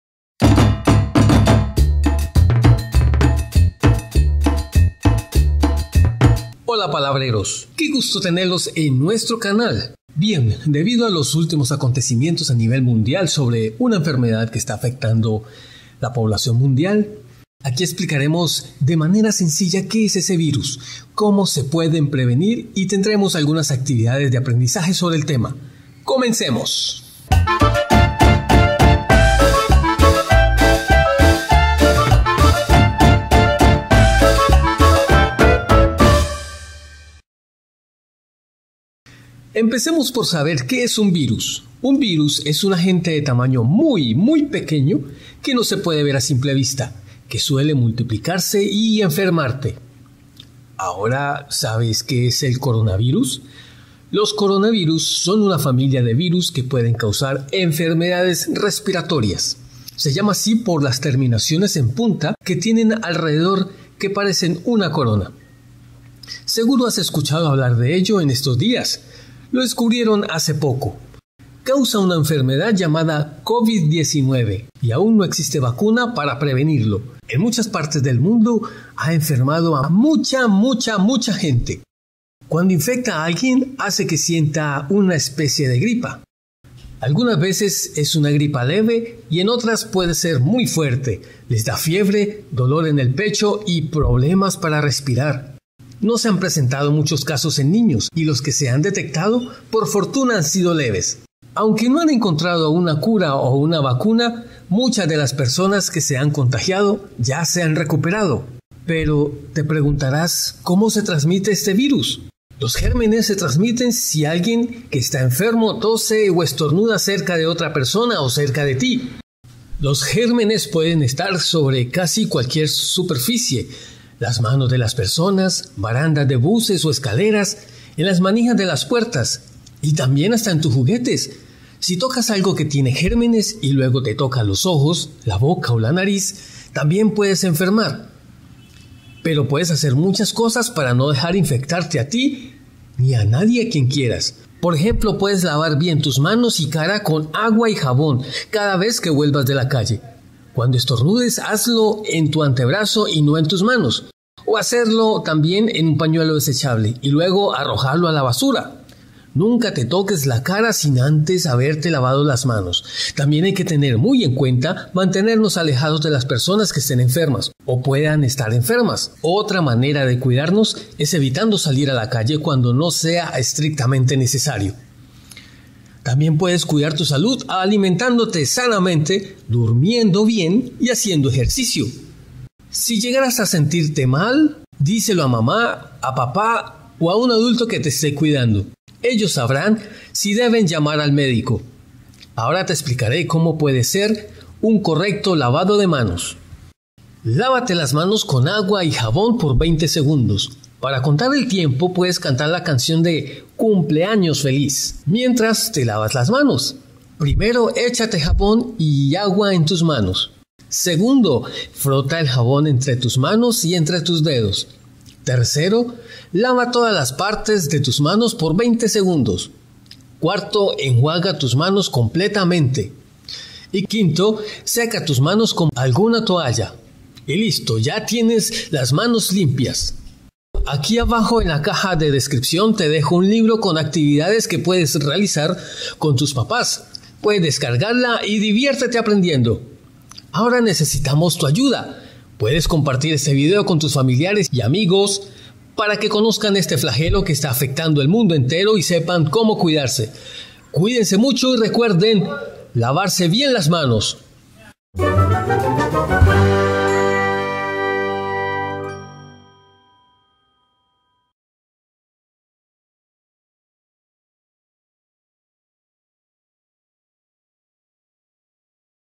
CORONAVIRUS explicado para niños | AbraPalabra
Si quieres seguir escuchando más cuentos como este...

Debido a los últimos acontecimientos  a nivel mundial sobre una enfermedad que está afectando a la población de todo el mundo, aquí explicaremos de manera sencilla qué es ese virus, cómo se pueden prevenir y tendremos algunas actividades de aprendizaje sobre el tema. Acá abajo dejaré el link donde puedes descargar el libro de actividades para niños, diviértete aprendiendo.